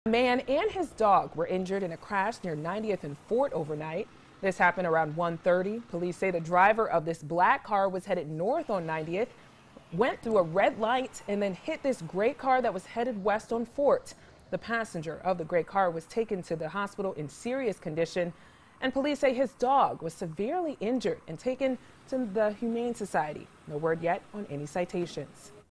Passenger, Dog Hurt In Two-Car Crash
90th and Fort